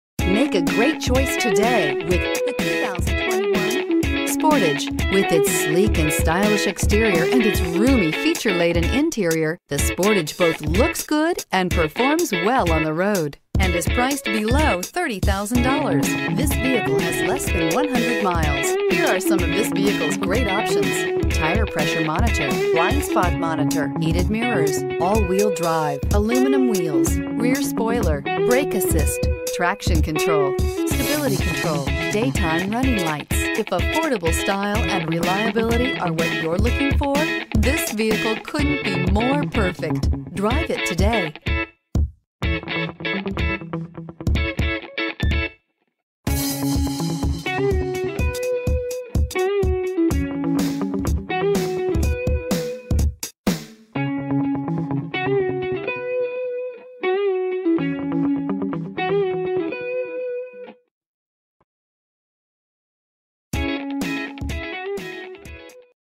2021 Kia Sportage K9888
2021 Kia Sportage S

Steel Gray 2021 Kia Sportage S AWD 6-Speed Automatic Electronic with Overdrive 2.4L I4 DGI DOHC 16V AWD.Recent Arrival! Combination of dealer discounts and manufacturer incentives. Price excludes tax, title, license and documentation fee.$2500 - Kia Customer Cash. Exp. 02/01/2021